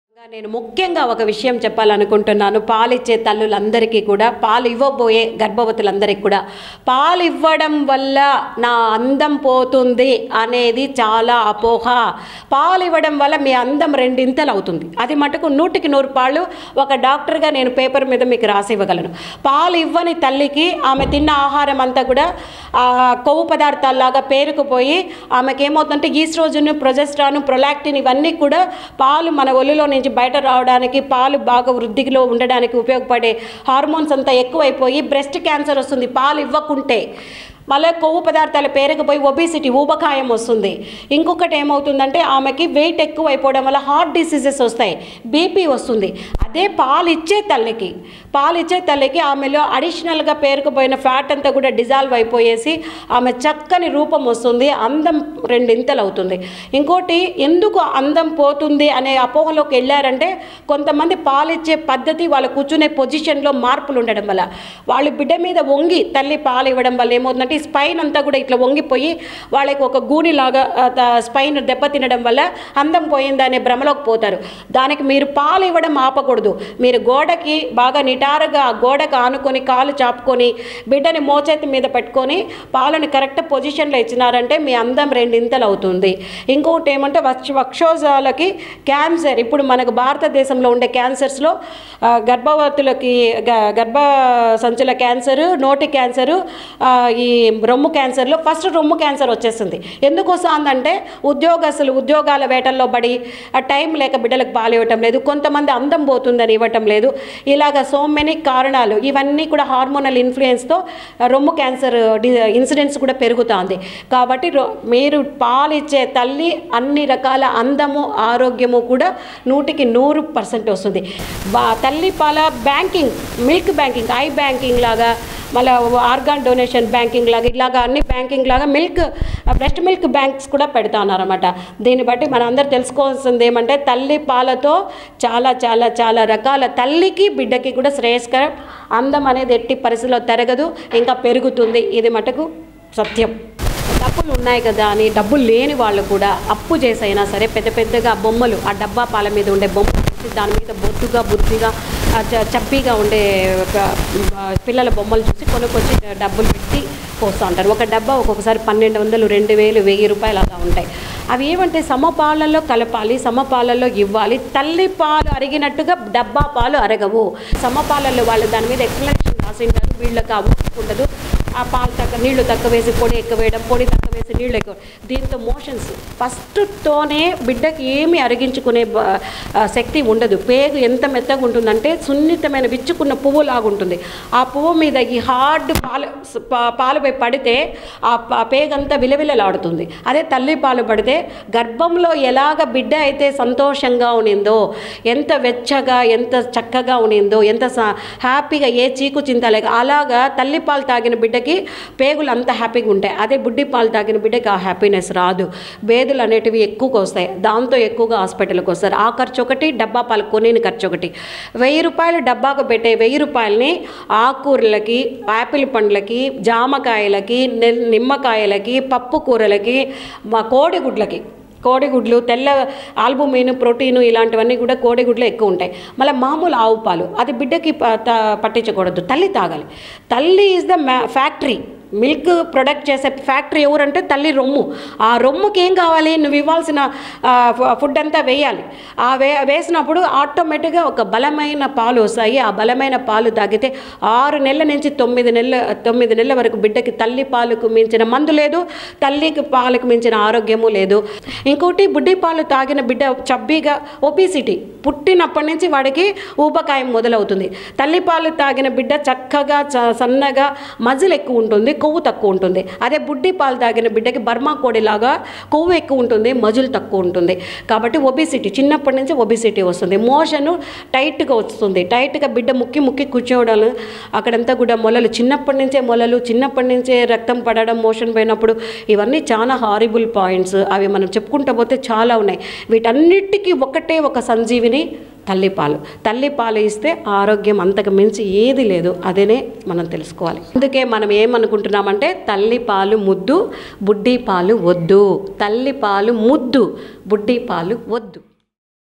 బిడ్డలకు పాలు ఇవ్వడం వల్ల , అందం రెట్టింపు అవుతుంది..ఇవ్వకపోతే బ్రెస్ట్ క్యాన్సర్ , గుండె జబ్బు.
బిడ్డలకు పాలు ఇవ్వడం వల్ల , అందం రెట్టింపు అవుతుంది.. బిడ్డలకు పాలు ఇవ్వకపోతే బ్రెస్ట్ క్యాన్సర్ , గుండె జబ్బులు , ఊబకాయం లాంటి జబ్బులొస్తాయి.. బిడ్డలకు పాలు ఇవ్వడంలో పొజిషన్ ముఖ్యం.. నెల్లూరు ప్రభుత్వ ఆసుపత్రి డాక్టర్ రోజారమణి తల్లిపాల వారోత్సవాల్లో అద్భుతమైన నిజాలు చెప్పారు.. వినండి.